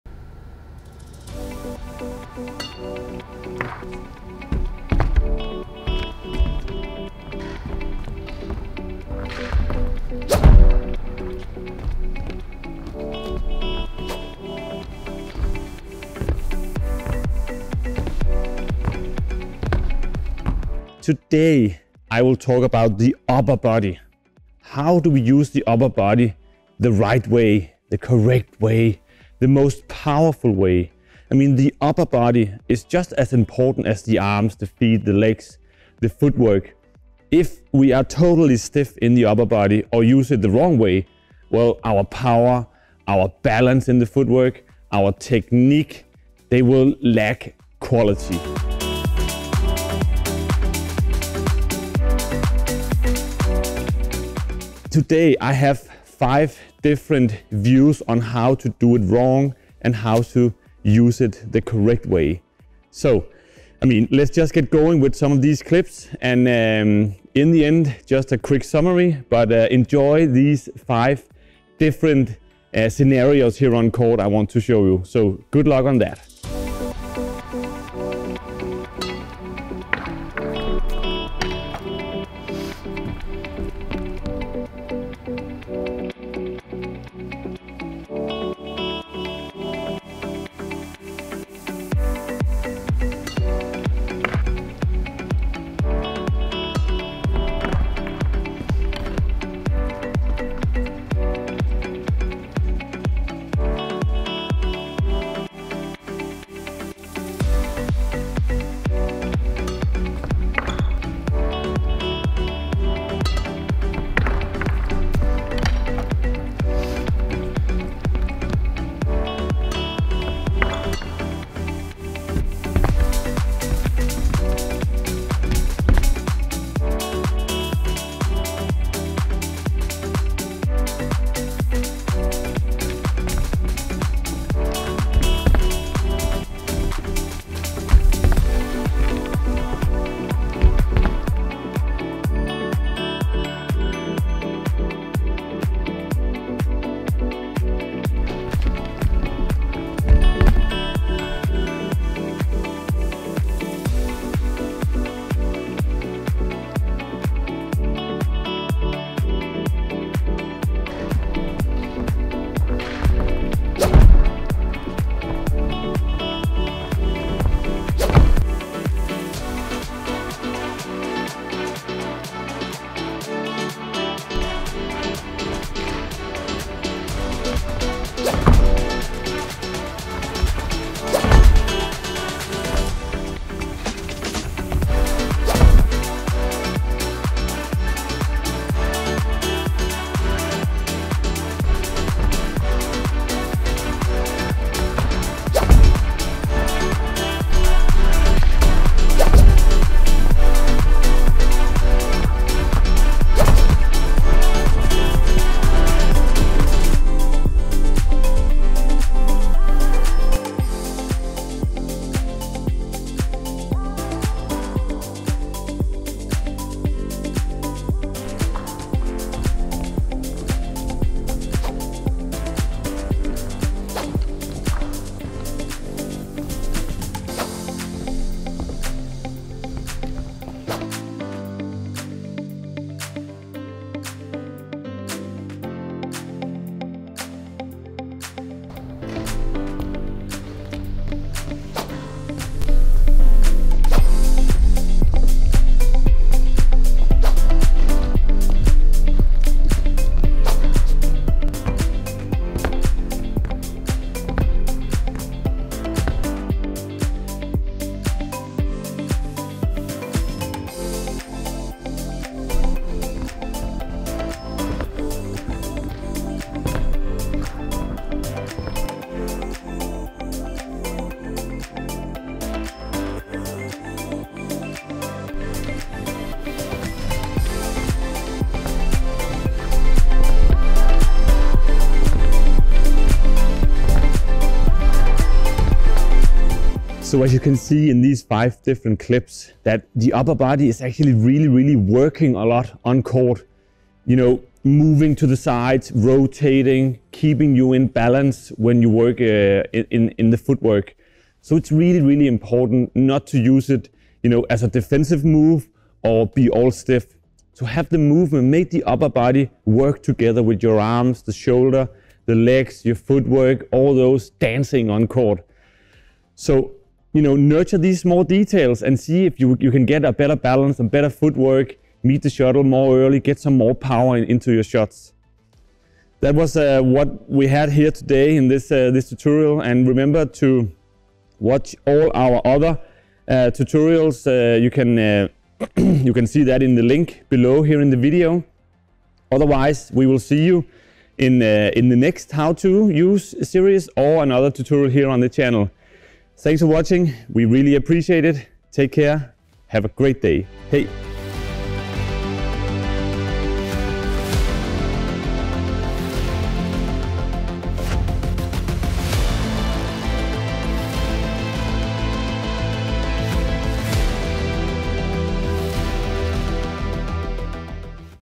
How to use the upper body - 5 moves biomechanics
How to use the upper body - 5 moves biomechanics. Welcome to this tutorial about how to use your upper body in badminton and how to move correctly for you to be faster, have better balance, make more variations, and hit with more power in your shots. The upper body is deeply connected to the footwork and the core. I show 5 wrong moves and then 5 correct ones, then you can see the difference in the technique.

The 5 moves are 👇🏻
• Low serve
• Net approach
• Scissor jump
• Overhead shot
• Forehand defense